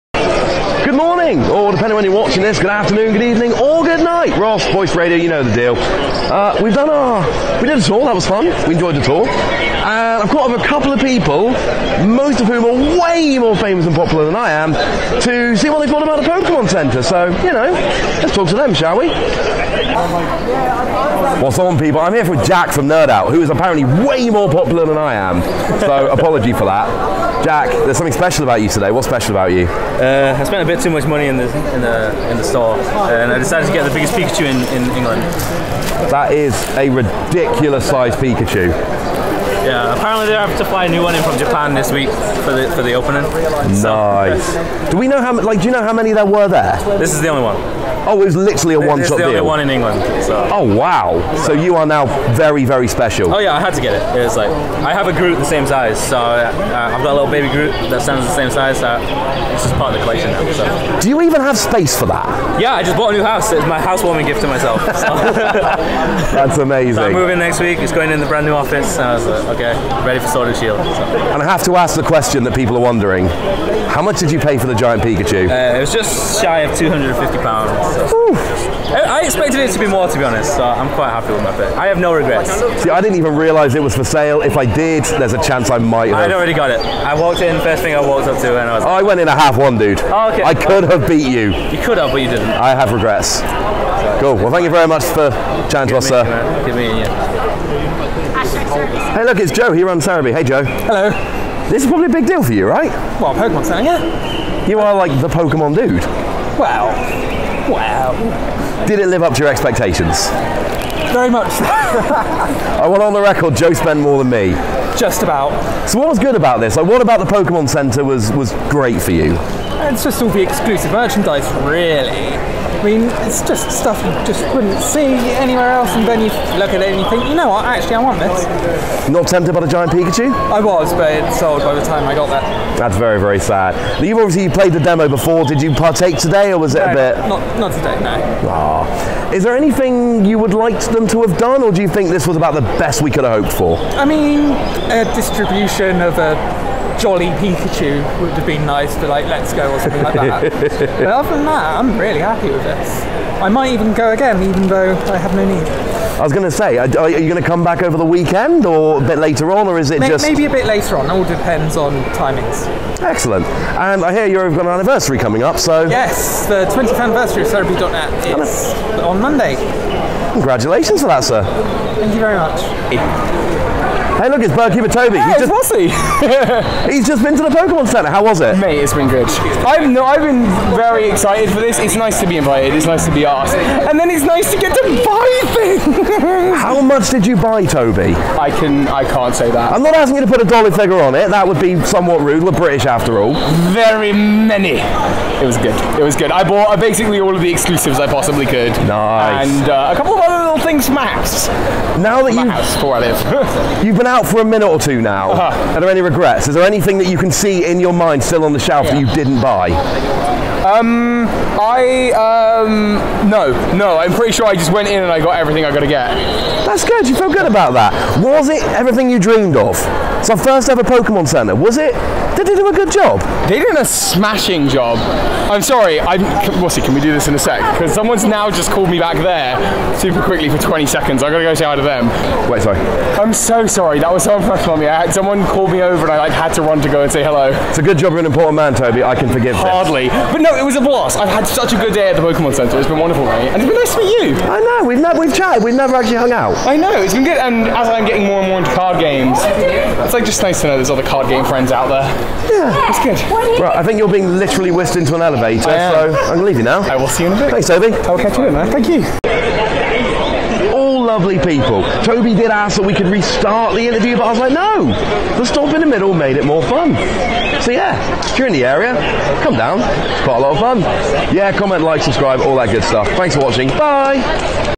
Pokemon Center London – Live Reactions!
The Pokemon Center London had a preview event today, I caught up with some people a LOT more popular than me to see what they thought!